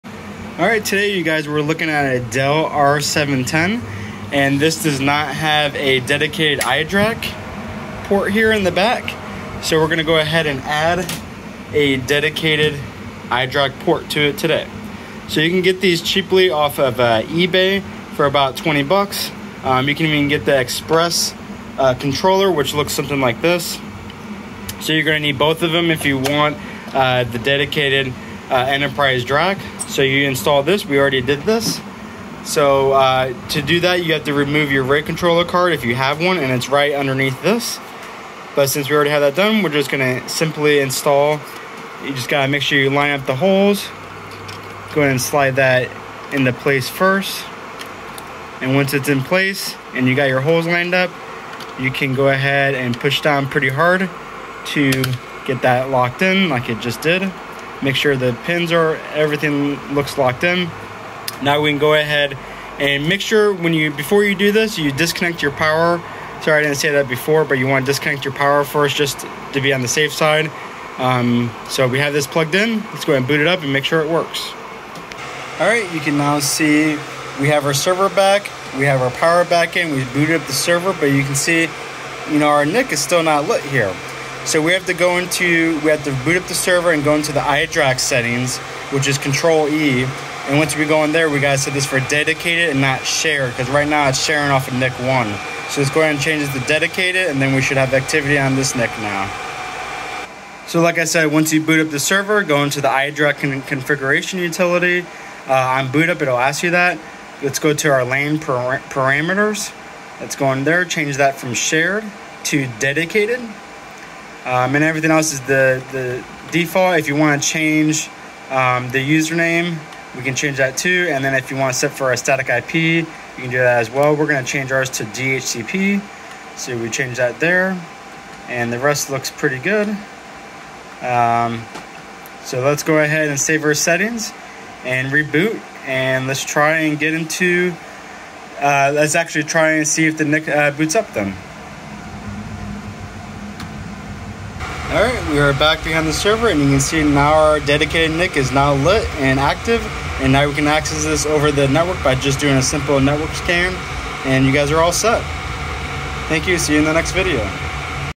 How to add Dedicated iDrac card to a Dell Server
In this video I will be showing you a Dell R710 server that does not have a Dedicated iDrac card in it and we will install one then show you how to get in and configure it to a static or DHCP IP. Then computer web browser like IE works best you can remotely manager your server.